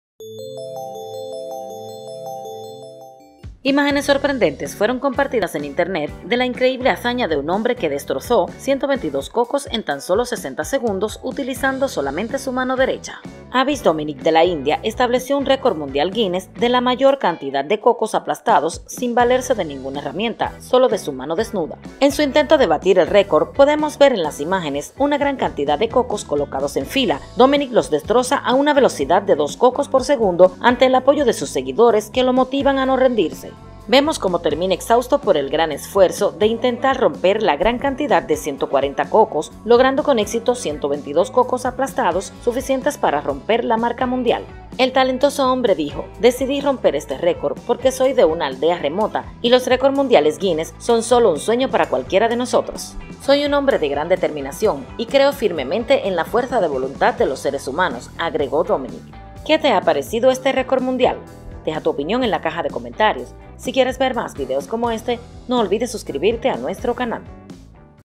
¡Increíble! Hombre Aplasta 122 Cocos Con Una Sola Mano
¡Increíble! Hombre Aplasta 122 Cocos Con Una Sola Mano. Imágenes sorprendentes fueron compartidas de la increíble hazaña de un hombre que destrozo 122 cocos en tan solo 60 segundos utilizando solo su mano derecha.

Abheesh Dominic de la India, estableció un récord mundial Guinness de la mayor cantidad de cocos aplastados sin valerse de ninguna herramienta, solo de su mano desnuda.

En su intento de batir el récord, podemos ver en las imágenes una gran cantidad de cocos colocados en fila, Dominic los destroza a una velocidad de dos cocos por segundo, ante el apoyo de sus seguidores que lo motivan a no rendirse.

Vemos como termina exhausto por el gran esfuerzo de intentar romper la enorme cantidad de 140 cocos, logrando con éxito 122 cocos aplastados, suficientes para romper la marca mundial.

El talentoso hombre dijo: "Decidí romper este récord porque soy de una aldea remota y los récords mundiales Guinness son sólo un sueño para cualquiera de nosotros".

"Soy un hombre de gran determinación y creo firmemente en la fuerza de voluntad de los seres humanos". Agregó Dominic.